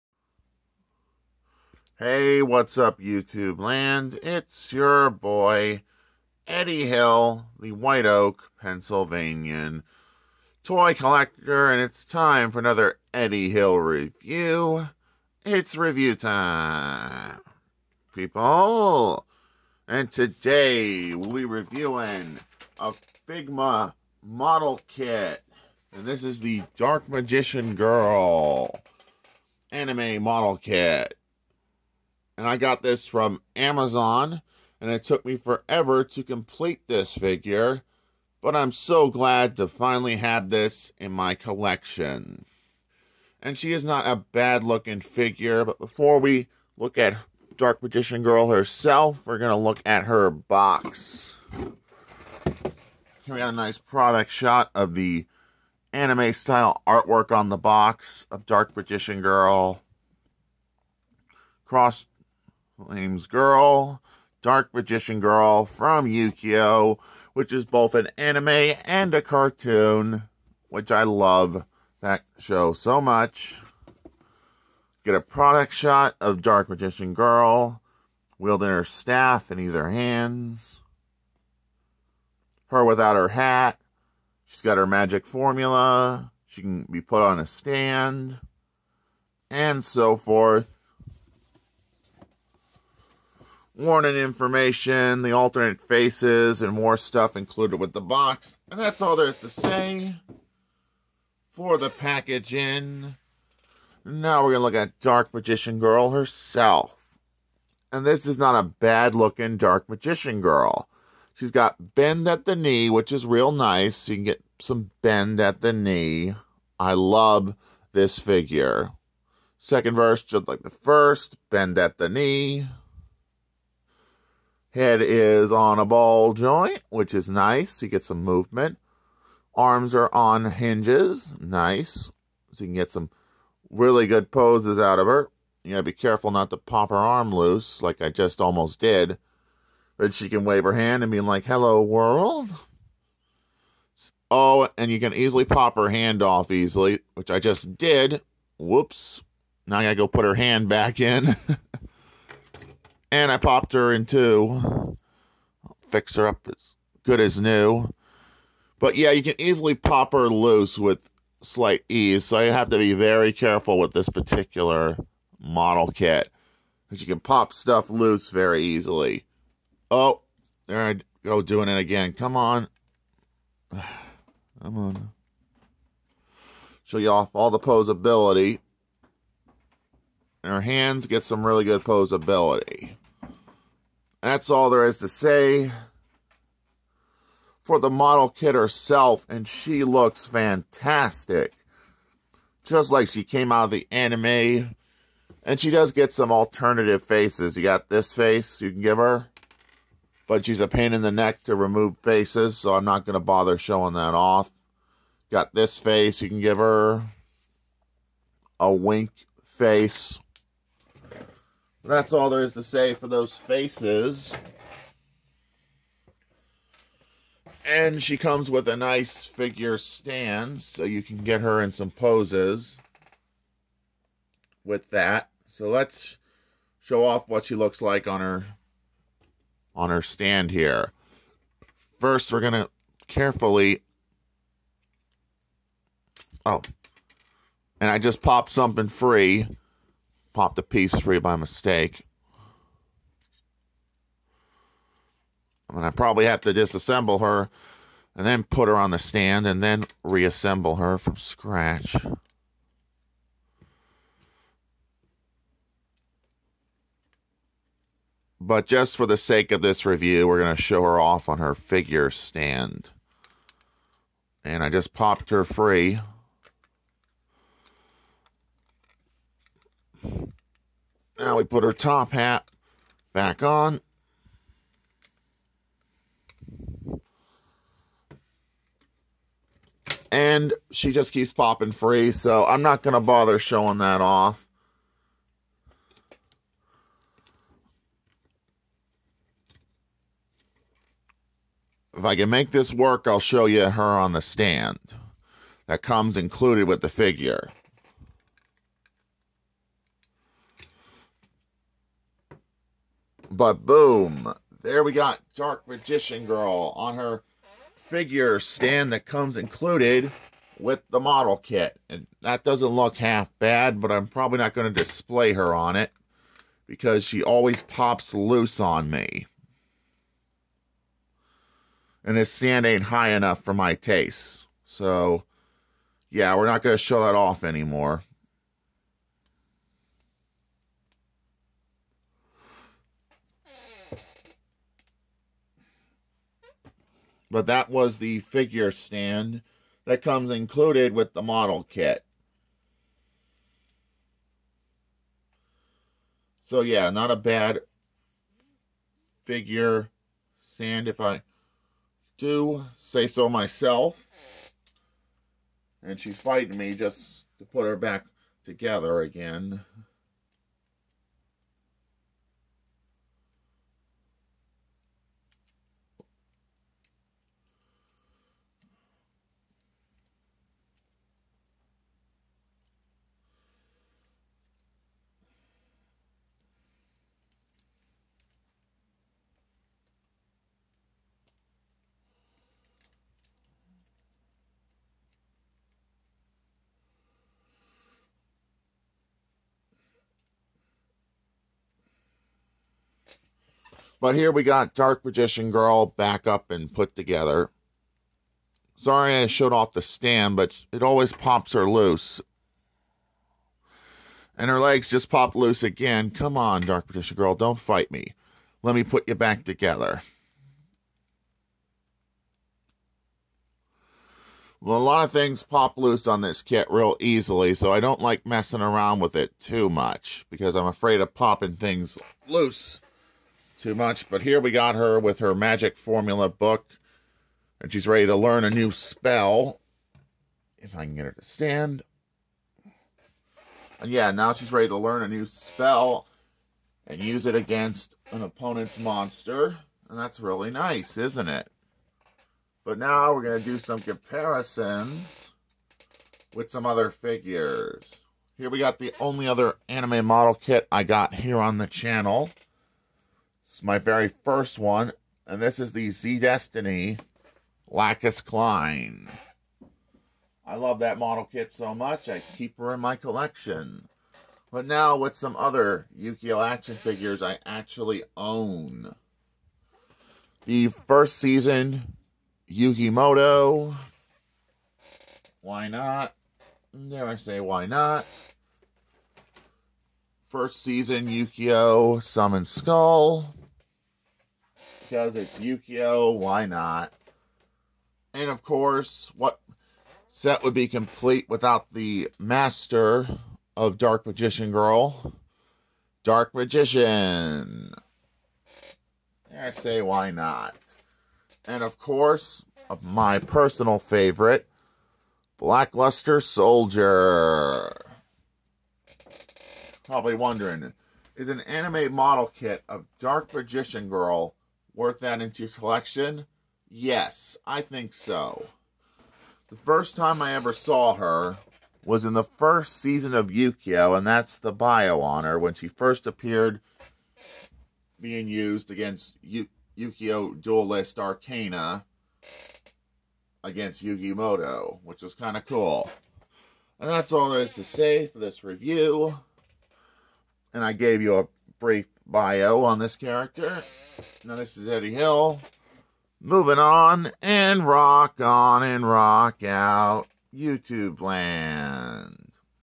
YUGIOH-DARK MAGICIAN GIRL-FIGMA MODEL KIT 2/26/23
EDDIE HILL REVIEWS:  YUGIOH-DARK MAGICIAN GIRL-FIGMA MODEL KIT  2/26/23